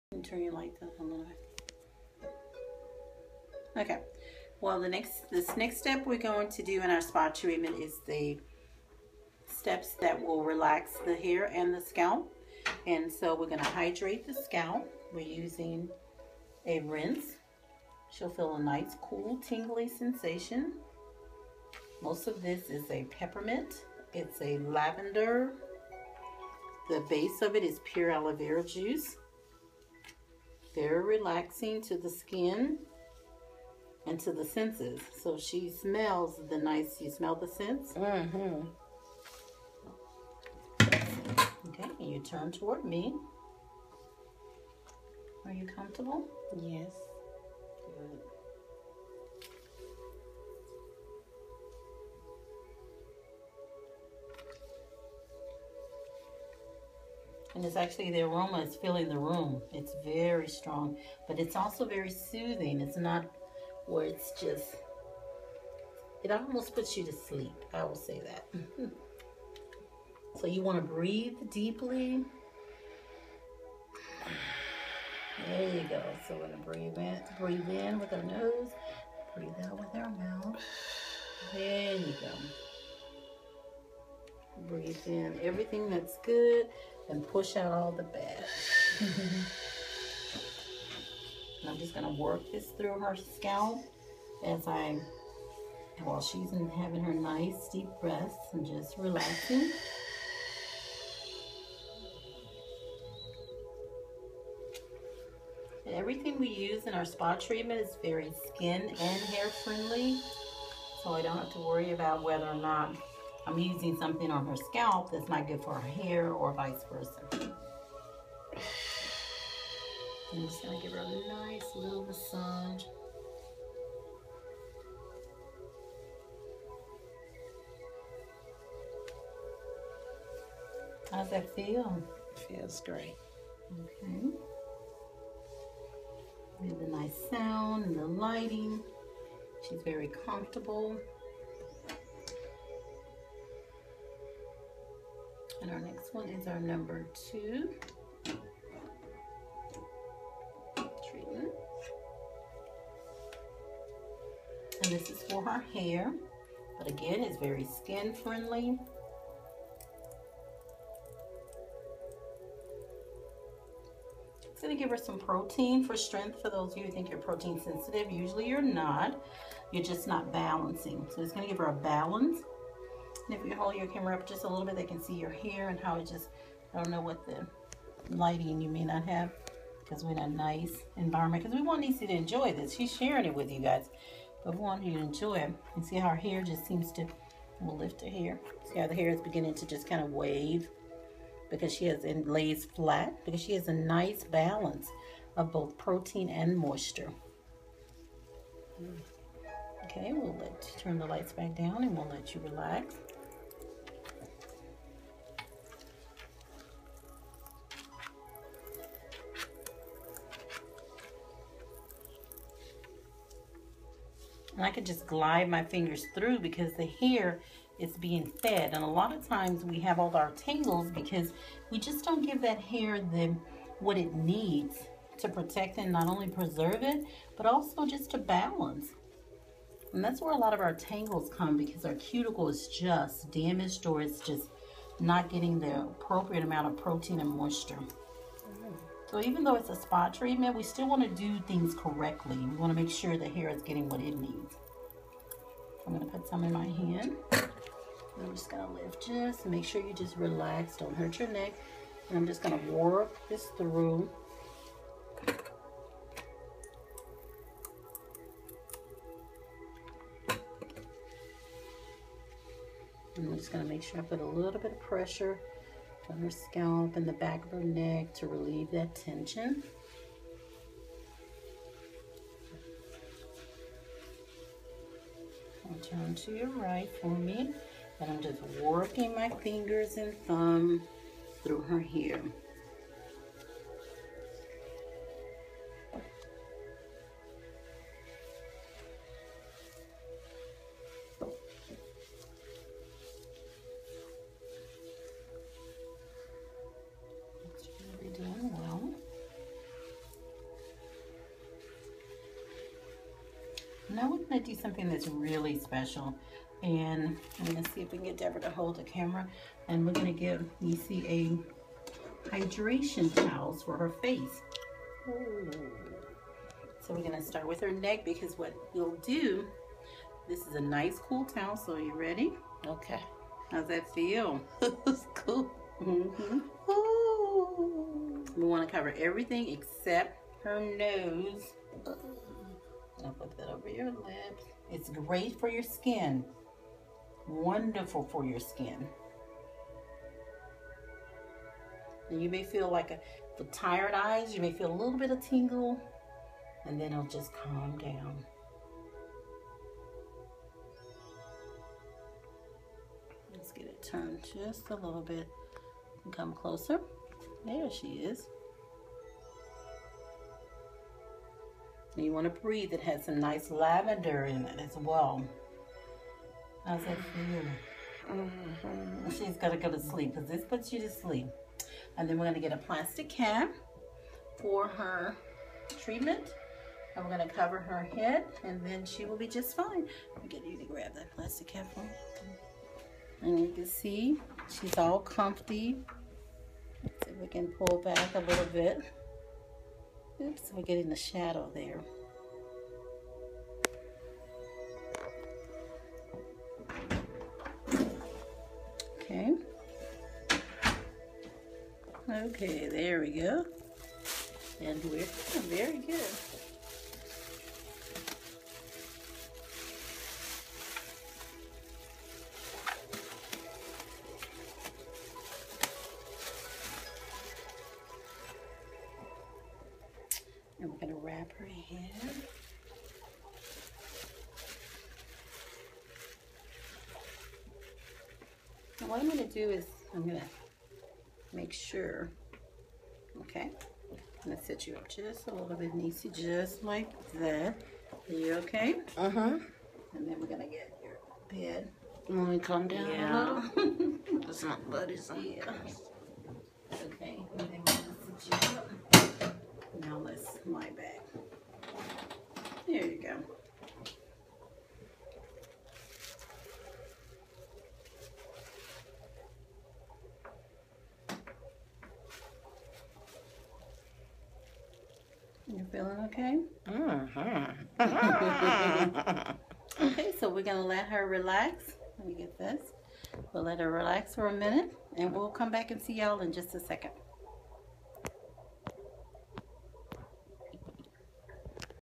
Lisa Akbari Signature Hair and Scalp Spa Treatment
A Step in Lisa Akbari's Spa treatment!!!
LeNiece Woods Blogger of N-TouchNews receiving her Birthday gift from Lisa Akbari Team